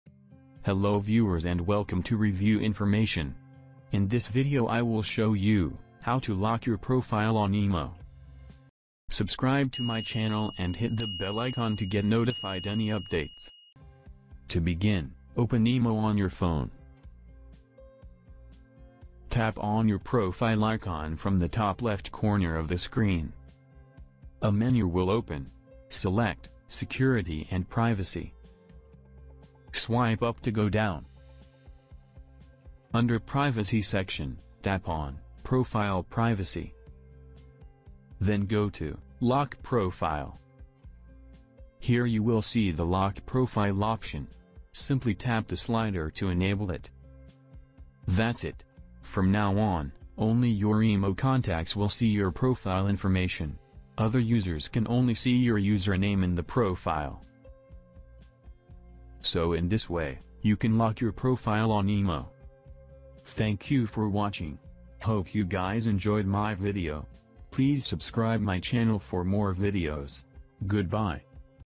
How to Lock Imo Profile from Unknown Person (New Update)
In this video I will show you, How to Lock Imo Profile from Unknown Person.

Open imo on your phone. Tap on your profile icon from the top left corner of the screen. A menu will open. Select security & privacy. Swipe up to go down. Under privacy section, tap on, profile privacy. Then go to, lock profile. Here you will see the lock profile option. Simply tap the slider to enable it. That's it. From now on, only your imo contacts will see your profile information. Other users can only see your username in the profile.